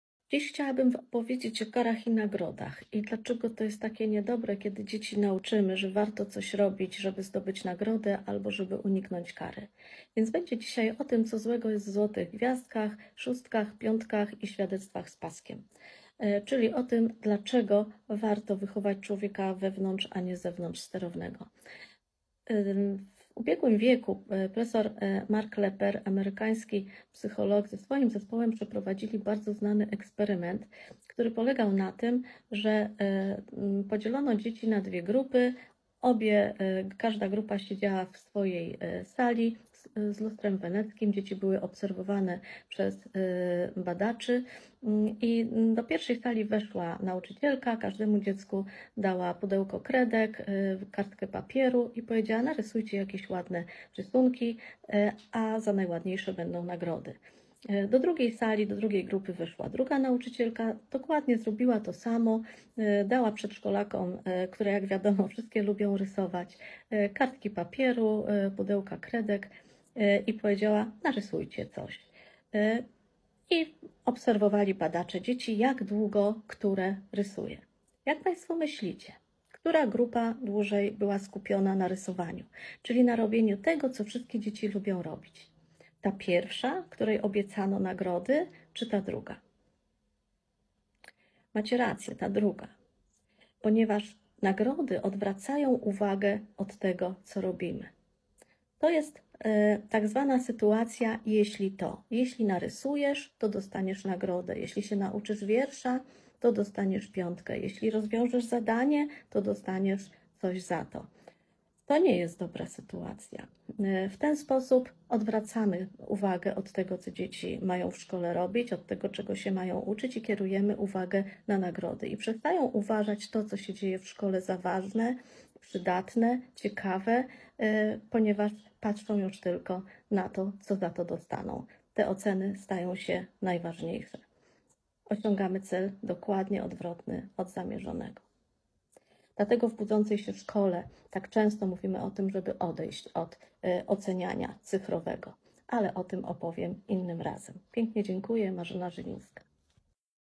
duet z @marzenazylinska , która opowiada o tym czego złego uczy się nas w
duet z @marzenazylinska , która opowiada o tym czego złego uczy się nas w szkole, ale to samo można odnieść do naszych miejsc pracy mobbing nierówności kary stres zastraszanie ucisk wyzysk tylko nas niszczą jako ludzi i społeczeństwo A my chcemy i lubimy dobrze pracować!